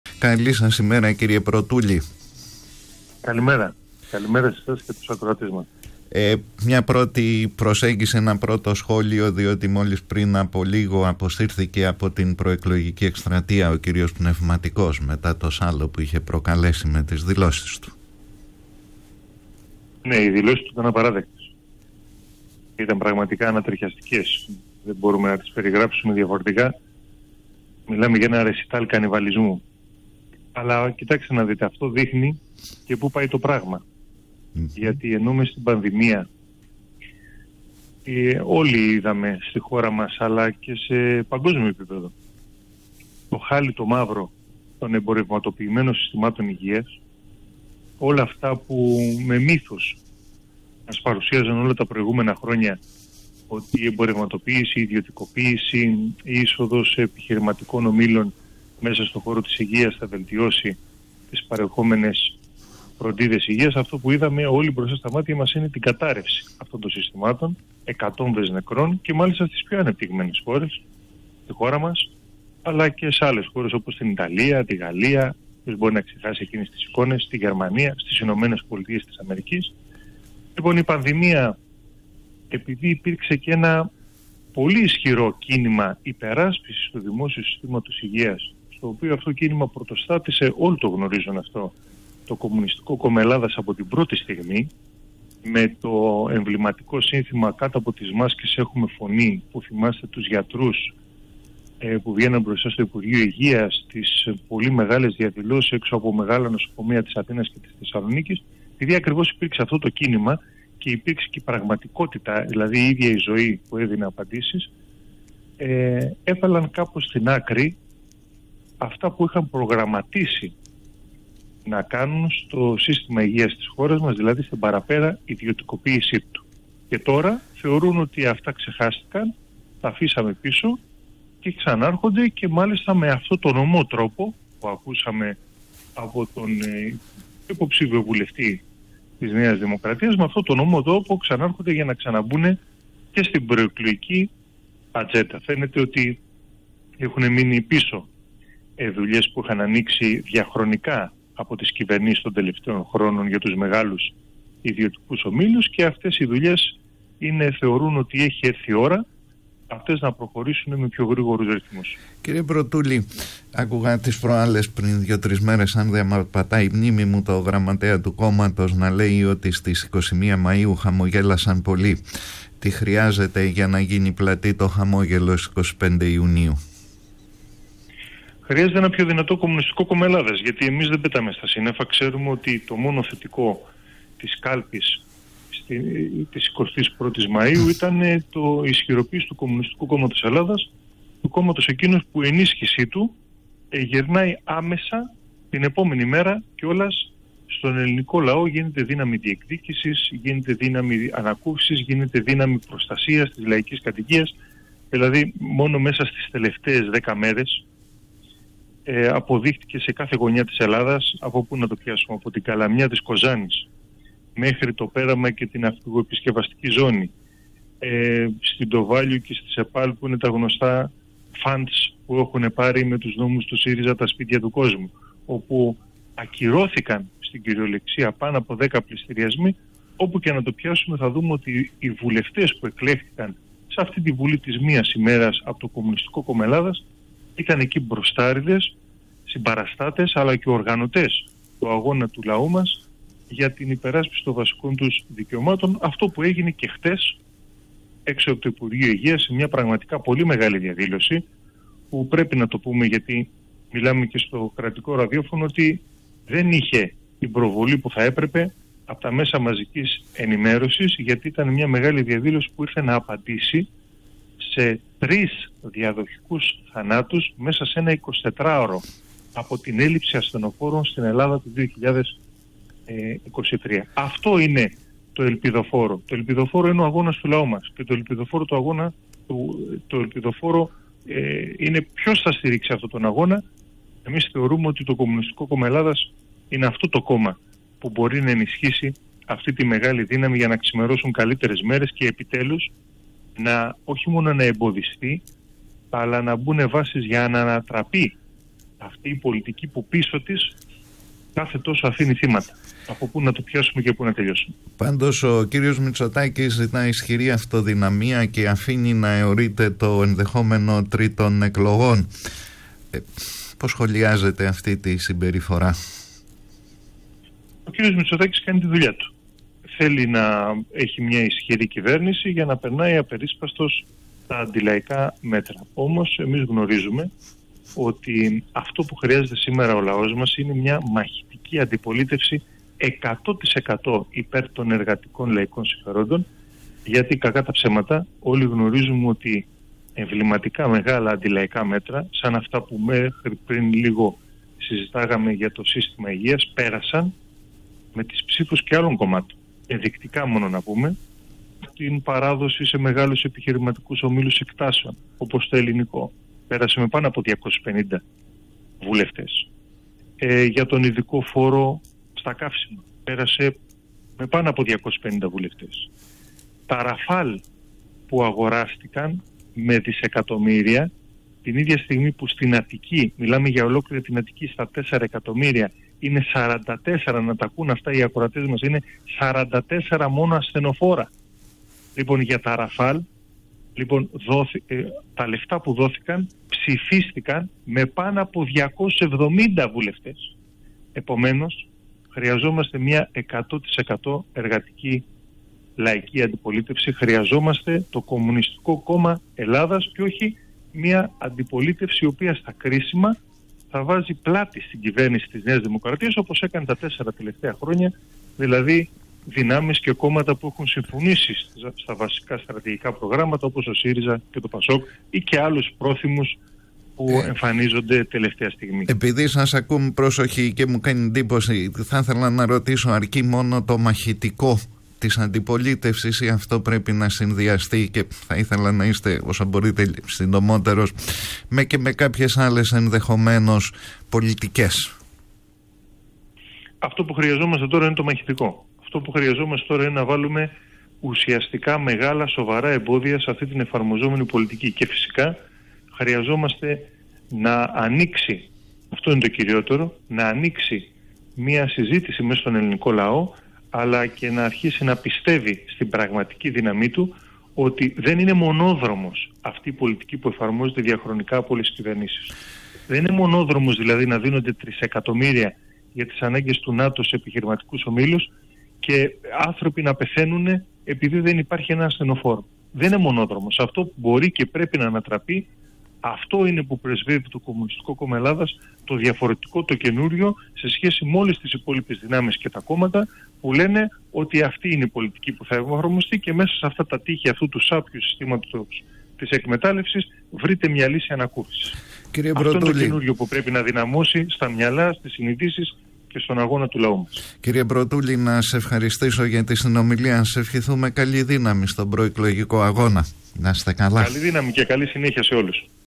Γ. Πρωτούλης: Η ψήφος στο ΚΚΕ γίνεται δύναμη ανακούφισης και διεκδίκησης του λαού
Η ψήφος στο ΚΚΕ γυρνάει άμεσα από την επόμενη κιόλας μέρα στον ελληνικό λαό, γίνεται δύναμη ανακούφισης, διεκδίκησης, προστασίας των δικαιωμάτων του λαού, επισήμανε ο Γιάννης Πρωτούλης, μέλος του ΠΓ της ΚΕ του ΚΚΕ και υποψήφιος περιφερειάρχης Αττικής με το ψηφοδέλτιο της «Λαϊκής Συσπείρωσης», μιλώντας στο Α’ Πρόγραμμα της ΕΡΑ το μεσημέρι της Παρασκευής 9 Ιούνη.

Πρόσθεσε ότι σε όλη την Ελλάδα οι βουλευτές του ΚΚΕ την επόμενη κιόλας μέρα της 25ης Μάη βρέθηκαν μπροστάρηδες και συμπαραστάτες στους αγώνες του λαού, από την Κοζάνη, το Πέραμα μέχρι την Αθήνα, στα «κοράκια» των funds ακυρώνοντας πάνω από 10 πλειστηριασμούς λαϊκών κατοικιών.

Υπογράμμισε πως αυτό που χρειάζεται σήμερα είναι μία μαχητική, λαϊκή αντιπολίτευση, 100% υπέρ των εργατικών συμφερόντων και αυτή μπορεί να την ασκήσει μόνο το ΚΚΕ. Τα άλλα κόμματα βάζουν πλάτη όταν πρόκειται να περάσουν κρίσιμα αντιλαϊκά μέτρα και υπενθύμισε πως με πάνω από 250 ψήφους πέρασαν τα νομοσχέδια για το Ελληνικό, για τον Ειδικό Φόρο στα καύσιμα, ενώ η αγορά των πολεμικών αεροσκαφών «Ραφάλ» ψηφίστηκε με πάνω από 270 ψήφους, την ώρα που στην Αττική κυκλοφορούν μόνο 44 ασθενοφόρα του ΕΚΑΒ.

Αυτή η πολιτική, που δίνει δισεκατομμύρια για τις ανάγκες του ΝΑΤΟ αλλά όχι για τις ανάγκες του λαού, δεν είναι μονόδρομος, σημείωσε χαρακτηριστικά και κάλεσε τον λαό να πιστέψει στη δύναμή του, να βάλει εμπόδια στα νέα αντεργατικά μέτρα που θα έρθουν, να εμπιστευτεί το ΚΚΕ γιατί αυτή η κατάσταση μπορεί και πρέπει να ανατραπεί.

Όσον αφορά τις δηλώσεις Πνευματικού, τις χαρακτήρισε ρεσιτάλ κανιβαλισμού που αποδεικνύουν τα σχέδια που έχουν τα αστικά κόμματα για τον δημόσιο χαρακτήρα της Υγείας.

Σημείωσε πως στην περίοδο της πανδημίας το κίνημα που αναπτύχθηκε με το σύνθημα «Κάτω από τις μάσκες έχουμε φωνή», όπου πρωτοστάτησε το ΚΚΕ, καθυστέρησε τα αντιλαϊκά σχέδια των αστικών κομμάτων για την παραπέρα εμπορευματοποίηση της Υγείας και τώρα επανέρχονται με τον πιο ωμό τρόπο, για να τα κάνουν πράξη, προκειμένου να ενισχύσουν τους ιδιωτικούς ομίλους και σε βάρος του δημόσιου χαρακτήρα της Υγείας.

Αναφέρθηκε στη χθεσινή μεγάλη κινητοποίηση στο υπ. Υγείας, όπου εργαζόμενοι και σωματεία καταδίκασαν αυτή την πολιτική που είχε ως αποτέλεσμα τρεις διαδοχικούς θανάτους μέσα σε ένα 24ωρο, λόγω των τραγικών ελλείψεων στο ΕΚΑΒ.